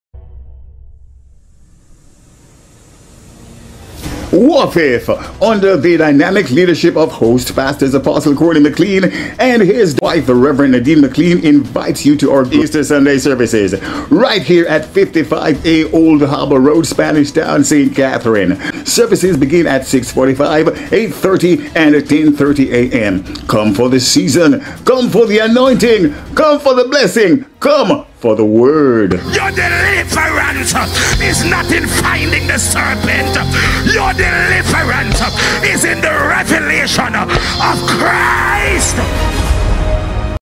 Easter Sunday Service
Join us this Sunday (April 9, 2023) @6:45am, 8:30am and 10:30am for our Easter Sunday Service. Come with an expectancy as we look to the Lord for the Supernatural.